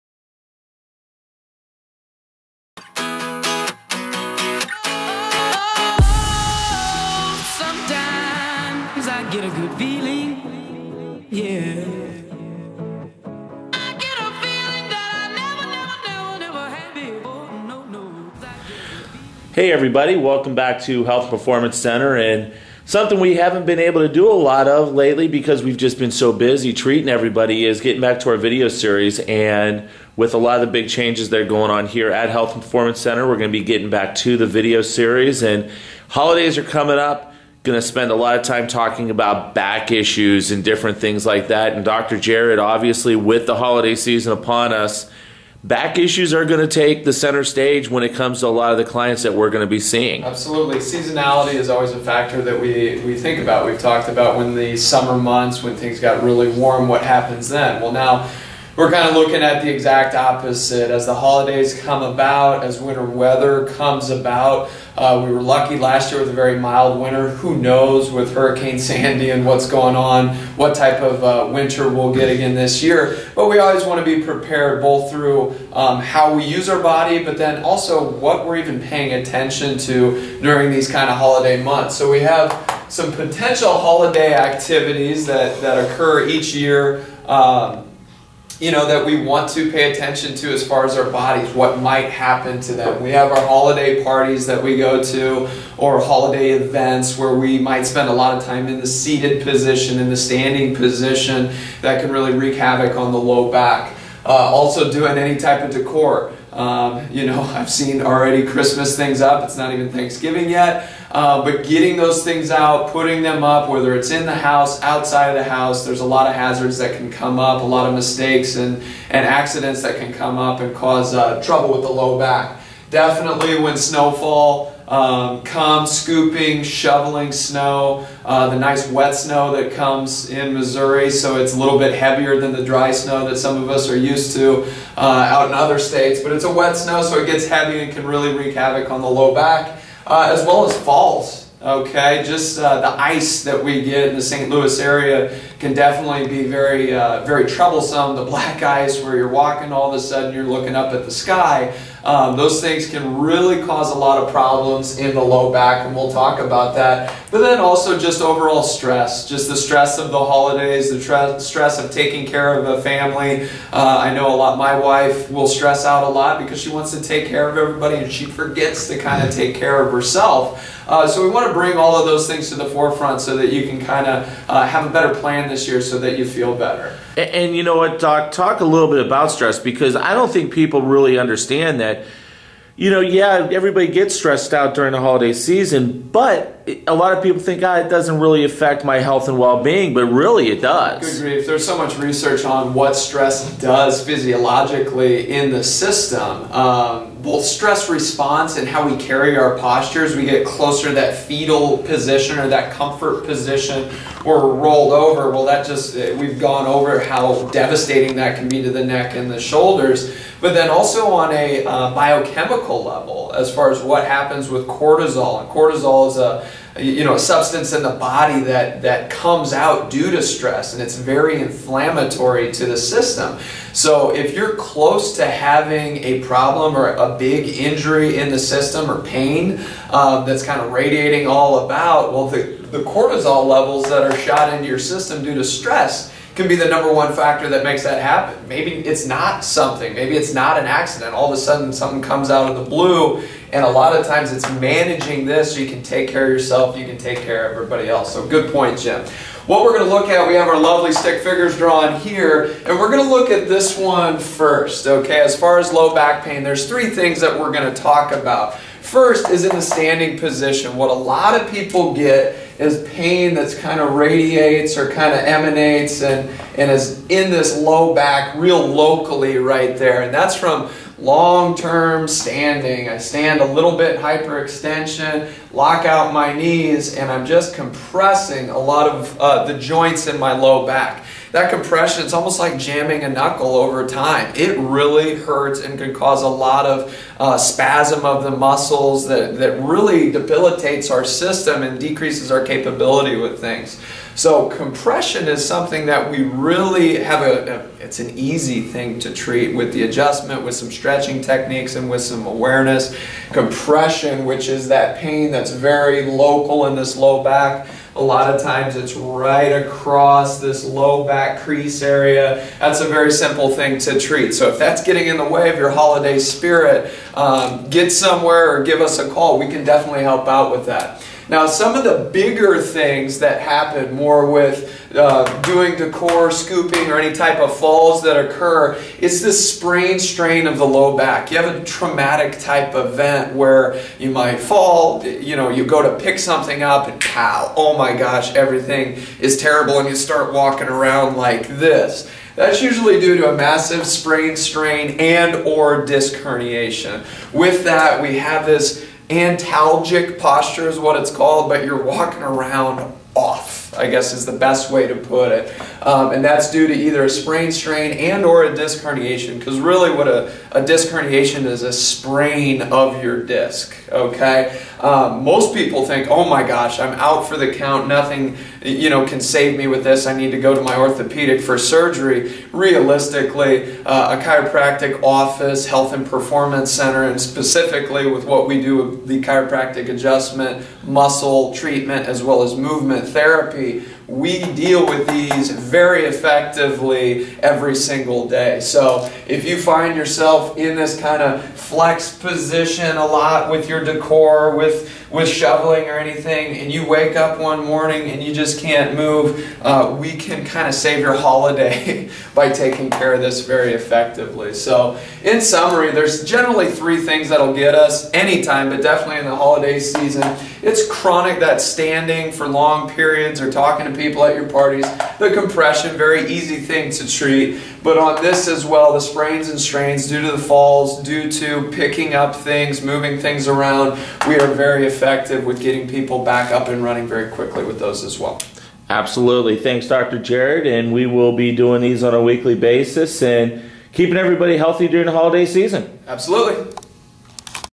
Low Back #1
Dr. Jared talks about low back pain. This is the first of our Low Back video series.